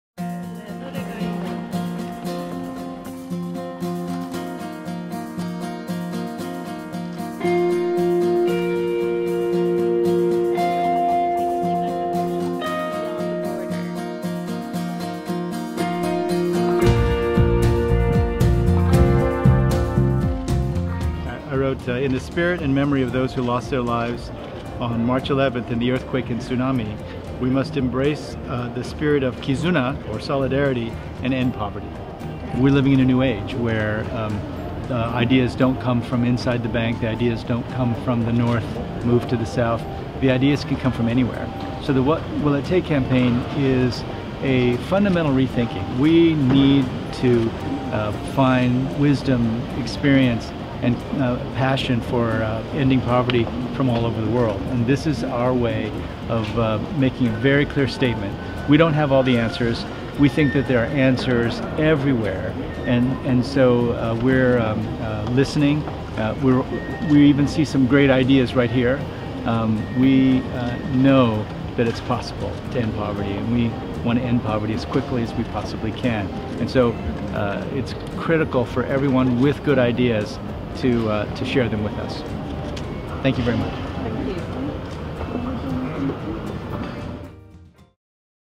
「貧困をなくすのに必要なことは」のボードを前に総裁が思いを語る
世界銀行の「貧困をなくすのに必要なことは(whatwillittake)」キャンペーンに寄せられた皆様からのメッセージを張り出したボードの前で、ジム・ヨム・キム世界銀行グループ総裁が思いを語りました。
2012年10月11日　ＩＭＦ世銀総会にて（東京国際フォーラム）

世界銀行東京事務所フェイスブックページ